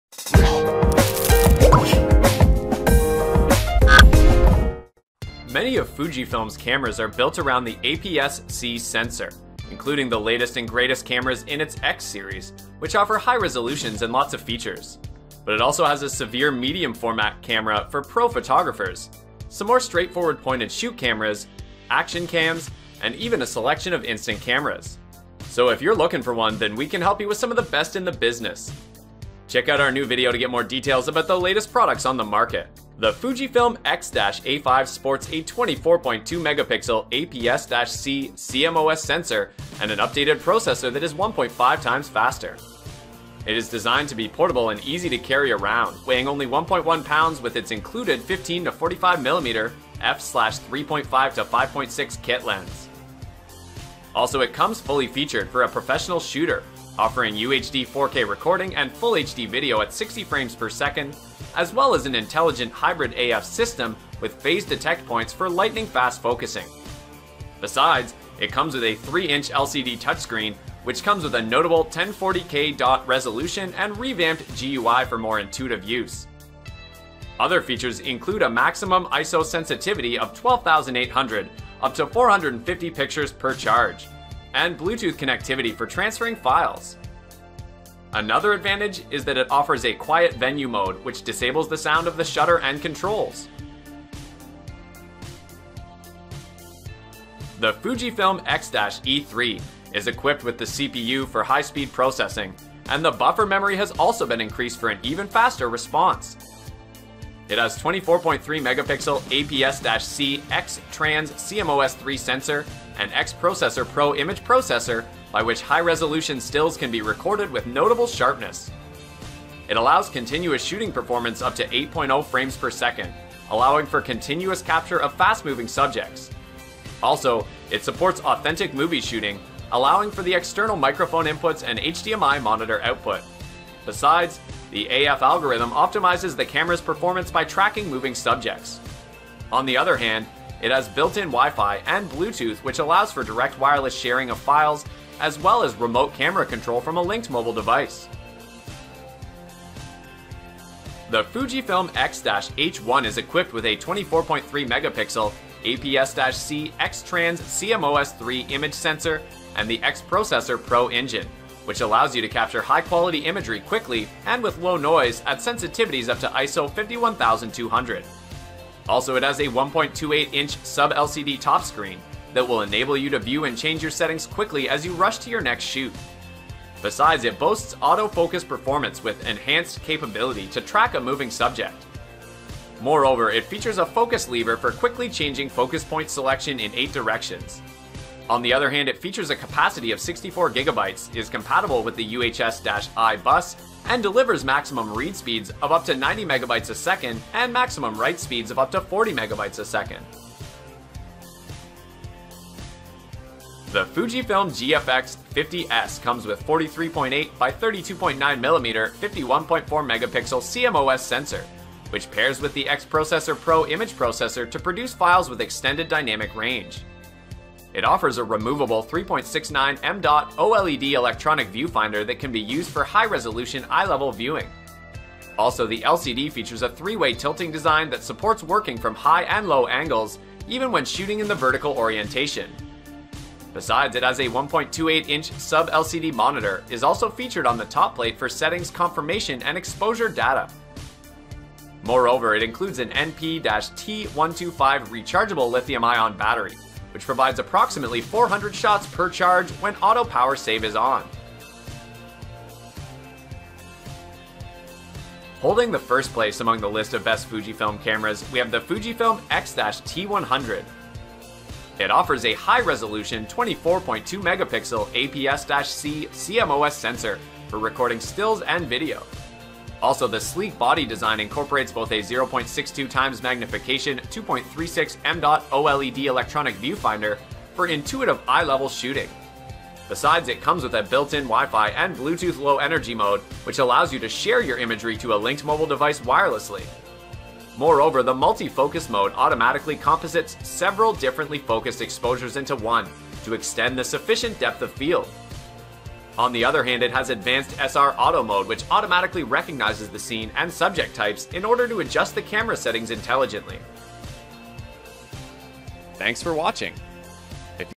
Best Fujifilm Cameras for Photography In 2020
1. Fujifilm X-T100
2. Fujifilm GFX 50S
3. Fujifilm X-H1
4. Fujifilm X-E3
5. Fujifilm X-A5